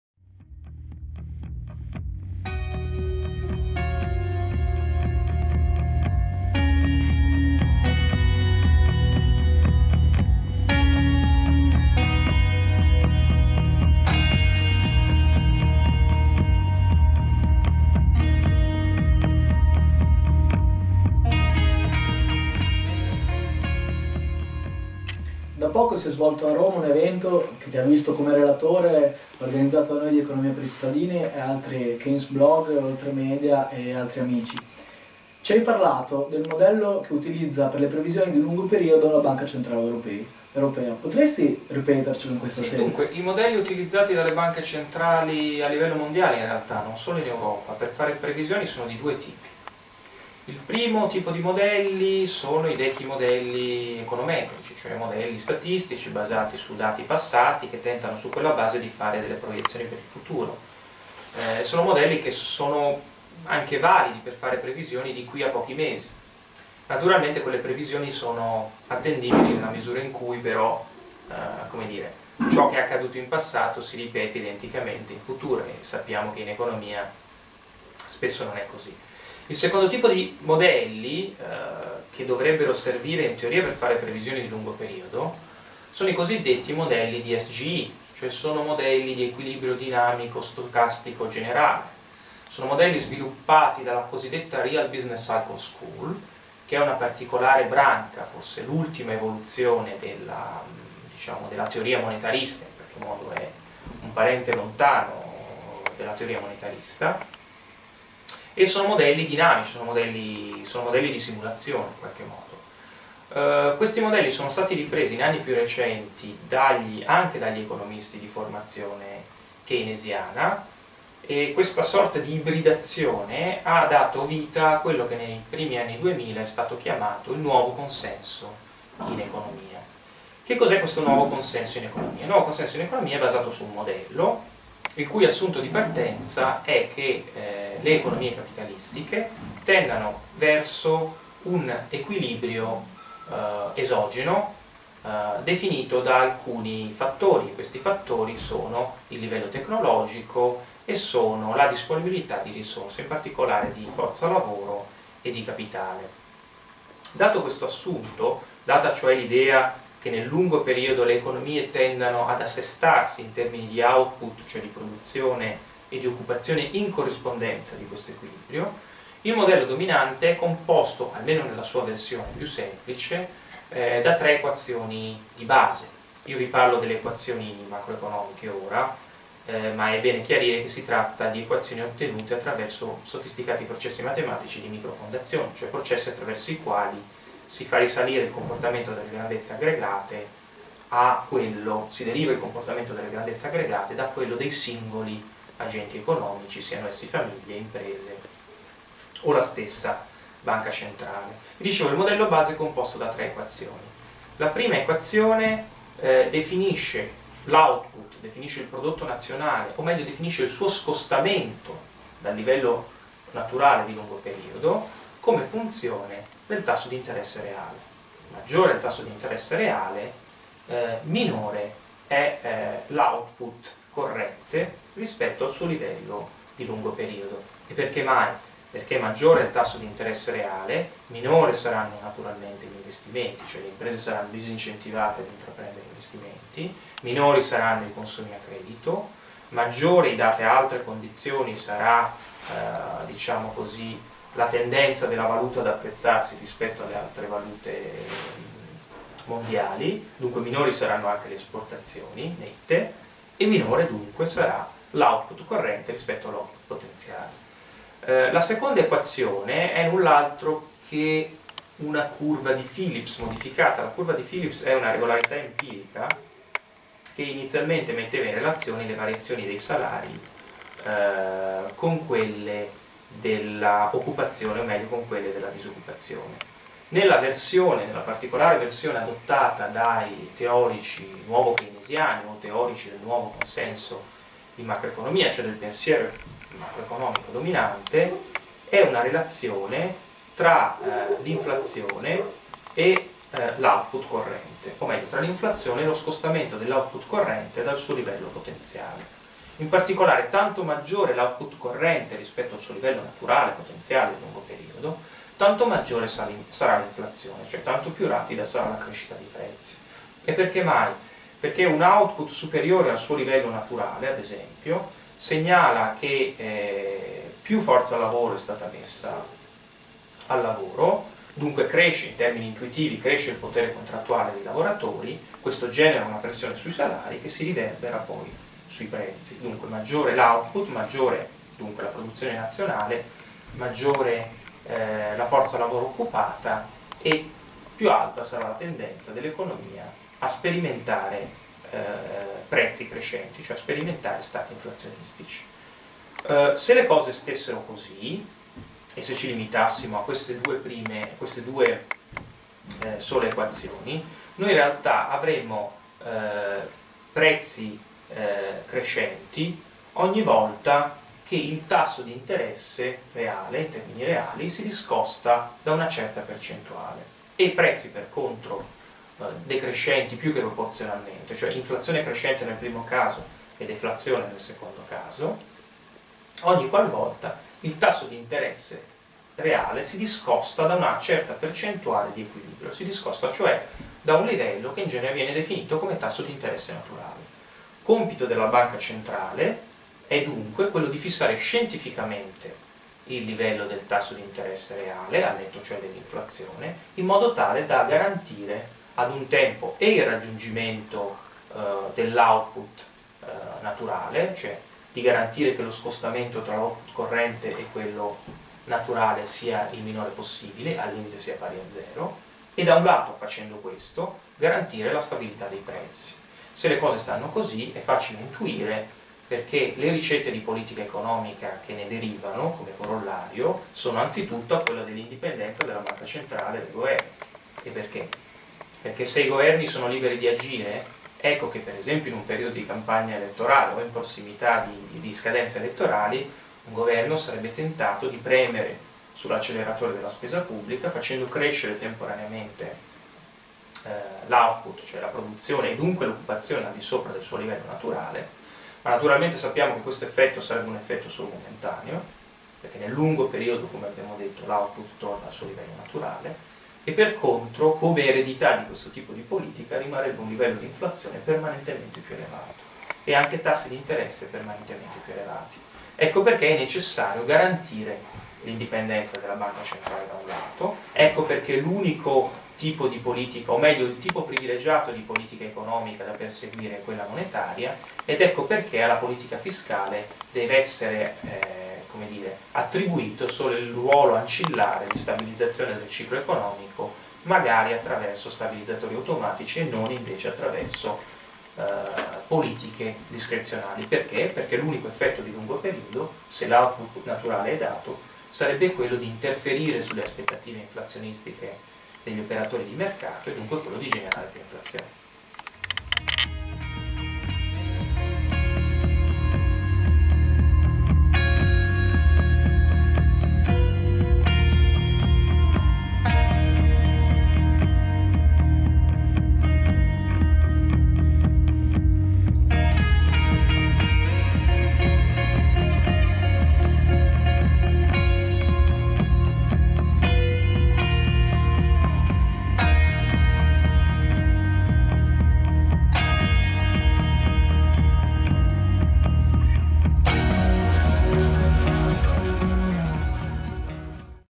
EPIC intervista MARCO V. PASSARELLA. 6) Il Modello di Previsione di Lungo Periodo della BCE (DSGE)
Marco Veronese Passarella
è Research Fellow presso la Leeds University Business School, ha ricevuto diversi premi per le sue ricerche ed è autore di numerosi saggi in campo economico.